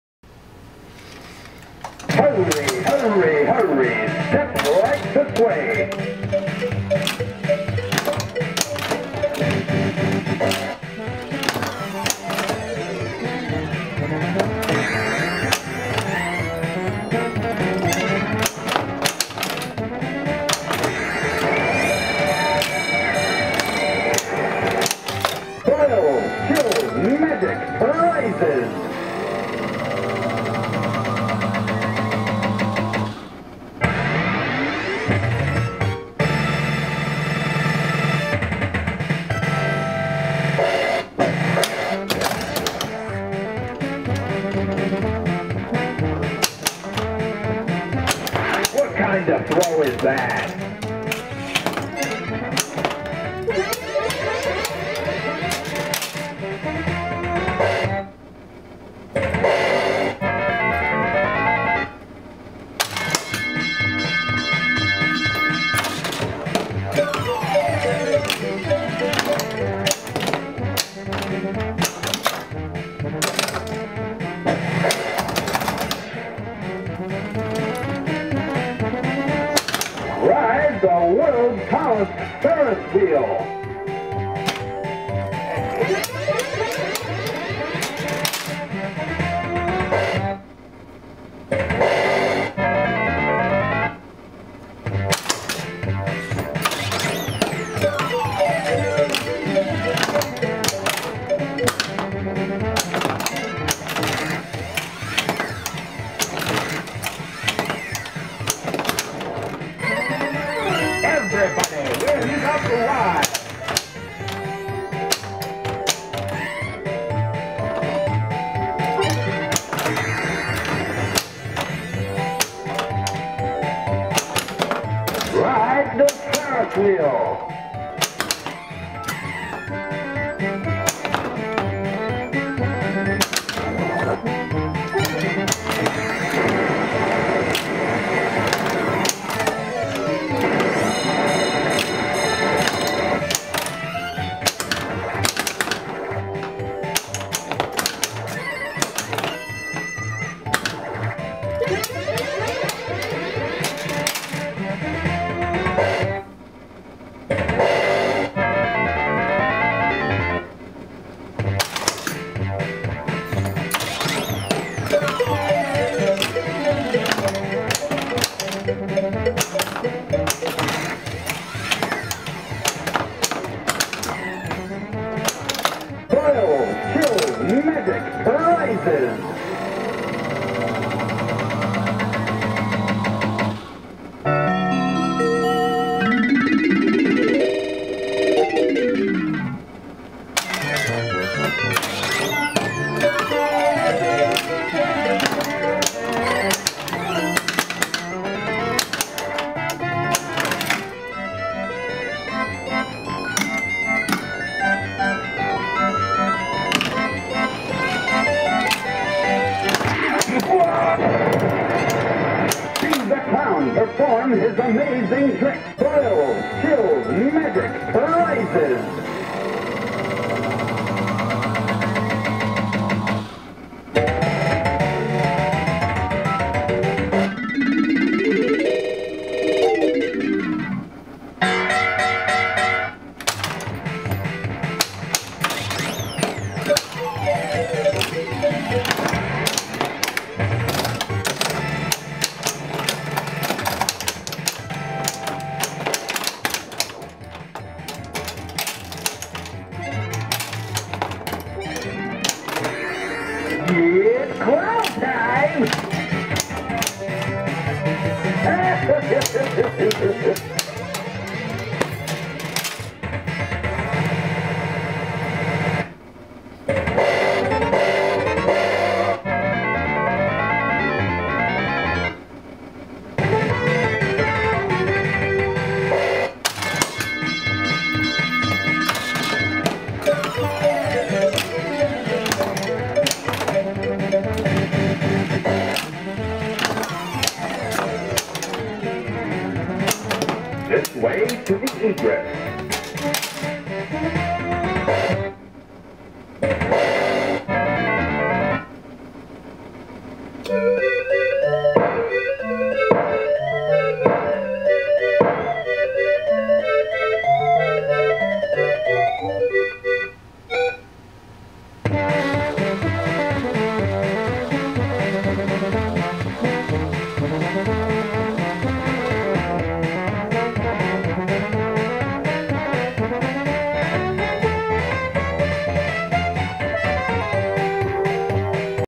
Hurricane Pinball Gameplay 2
Design by:   Barry Oursler
Art by:   John Youssi, Python Anghelo
Mechanics by:   Gerald Hedberg
Sound by:   Paul Heitsch
Software by:   Mark Penacho

Freshly shopped with upgraded speakers. As you can see, I'm not very good at this one.